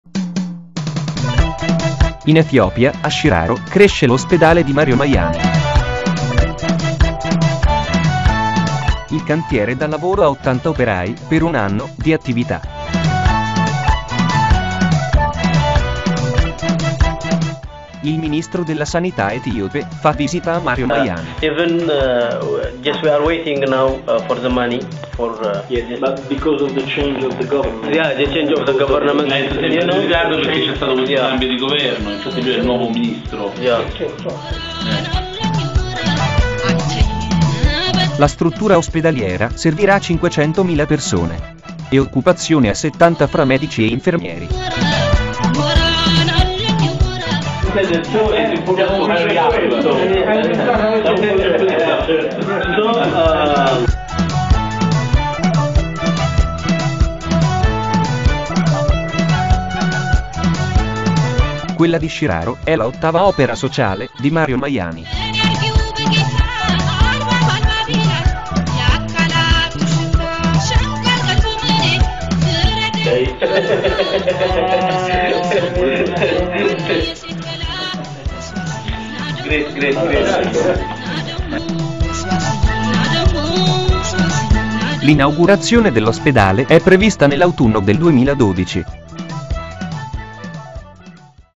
Visita del ministro etiope a Caldana
8 dicembre 2011 il ministro della sanità etiope visita Mario Maiani per fare  il punto dell'ospedale in costruzione a Sheraro in Abissinia.